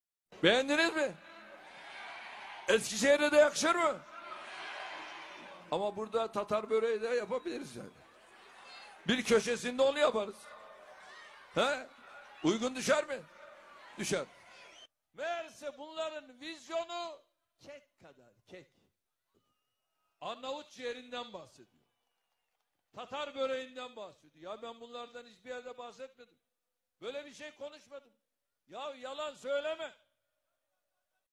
Erdoğanın Kendini son Yalanlaması ||| Tatar Böreği
eskişehir mitingide tatar böreğinden bahseden erdoğan aradan 5 gün sonra istanbul yenikapı mitinginde tatar böreğinden bahsetmedim dedi takdir sizin